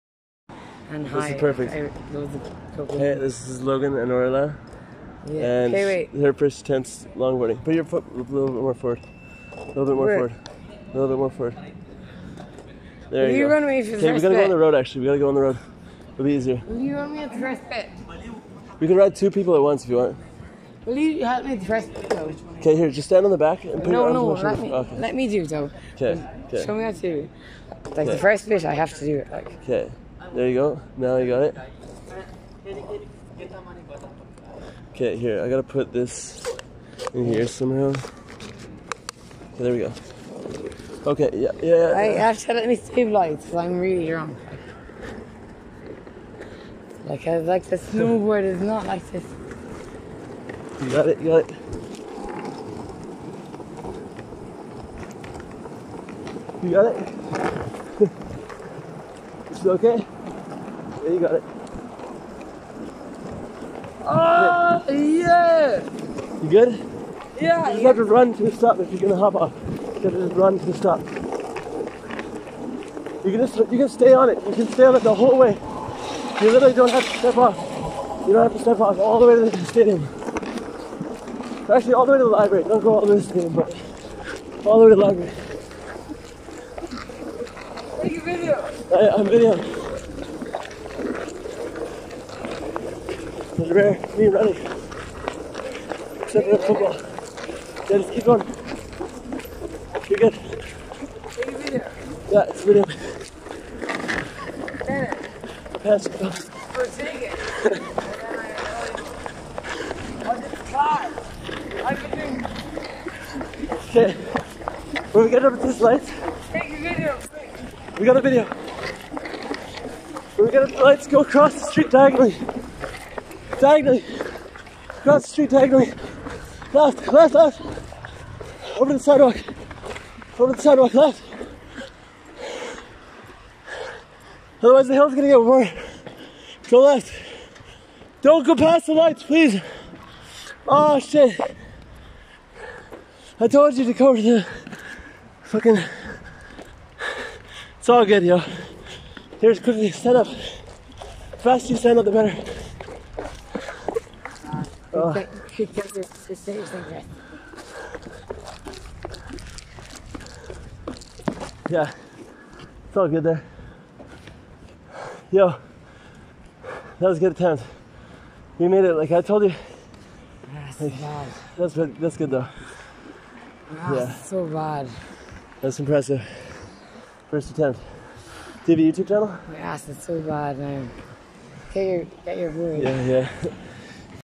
Big Time Longboarding First Try
Orla McDonagh and Logan Howls Down 2 Get It On My Longboard